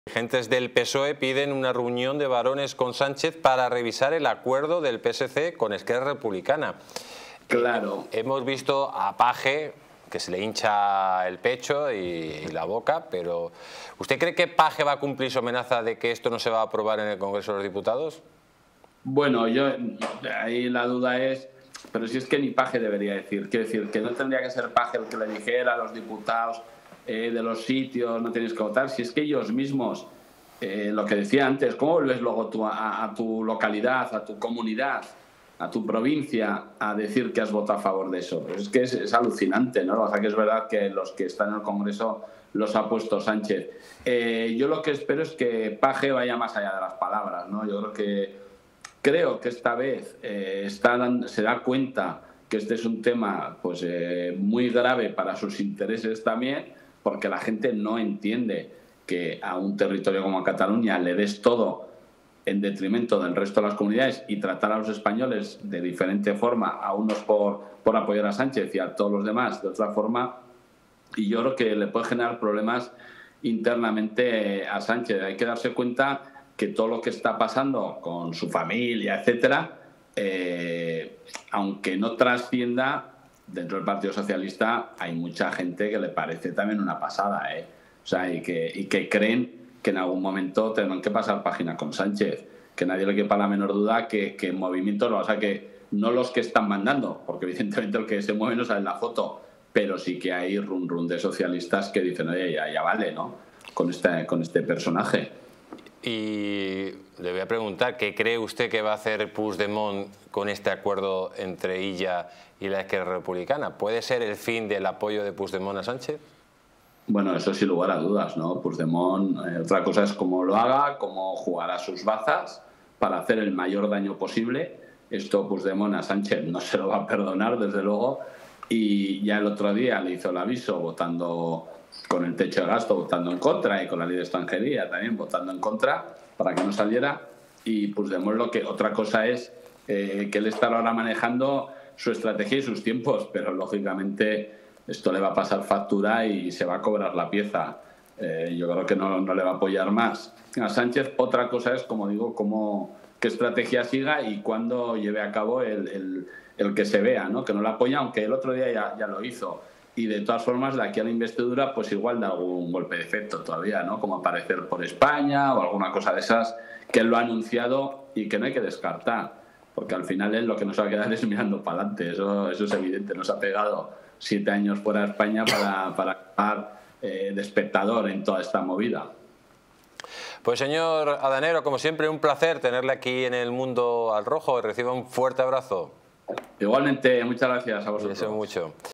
🔴Carlos García Adanero🔴el PSOE puede saltar por los aires, nadie quiere el pacto de Sánchez y ERC
En esta entrevista exclusiva, Carlos García Adanero habla sobre la situación actual del PSOE y cómo el partido podría estar al borde del colapso. ¡No te pierdas este análisis en Distrito TV!distritotv